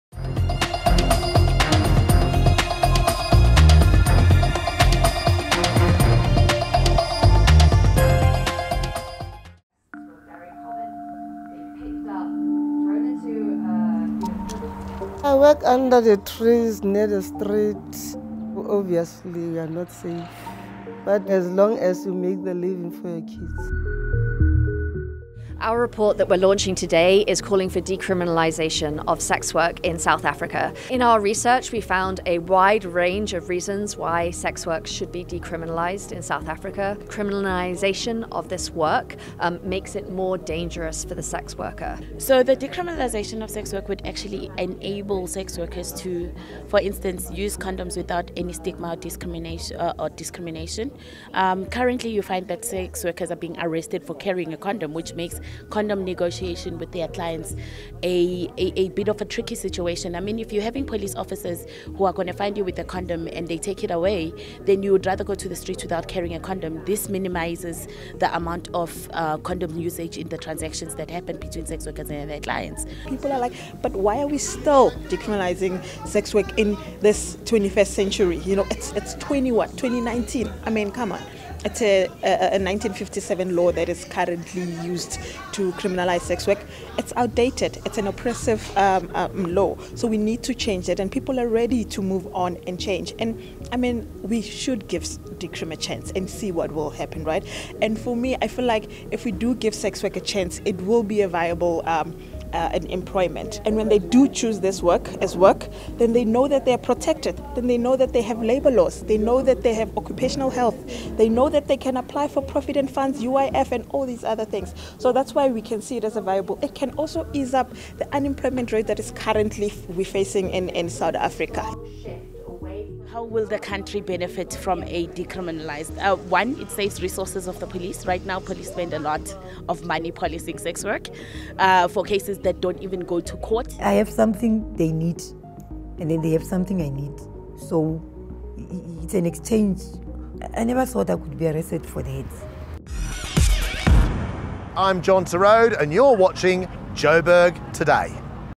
THE ISSUE - SEX WORK REPORT
On August 7th, Human Rights Watch, together with the Sex Workers Education & Advocacy Task Force (SWEAT), launched a new report detailing why sex work should be decriminalised.